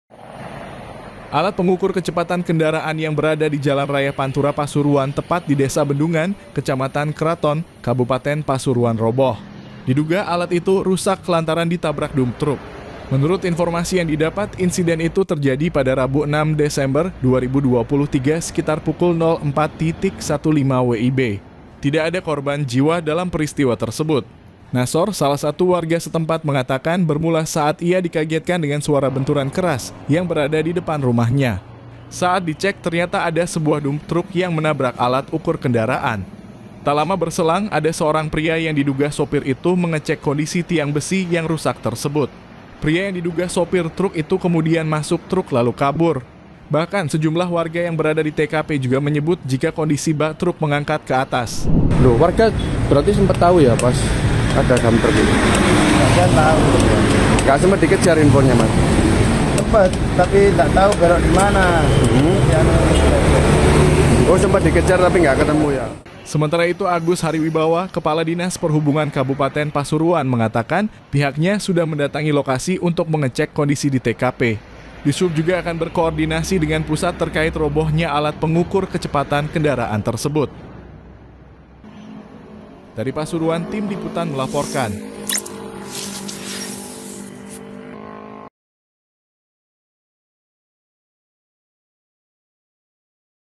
Diseruduk Dump Truck, Pengukur Kecepatan di Kraton Roboh
Kraton (WartaBromo.com) – Alat pengukur kecepatan kendaraan yang berada di Jalan raya Pantura Pasuruan tepat di Desa Bendungan, Kecamatan Kraton, Kabupaten Pasuruan roboh. Diduga, alat itu rusak lantaran ditabrak dump truk.

Menurut informasi yang didapat, insiden itu terjadi pada Rabu (6/12/2023) sekitar pukul 04.15 . Tidak ada korban jiwa dalam peristiwa tersebut.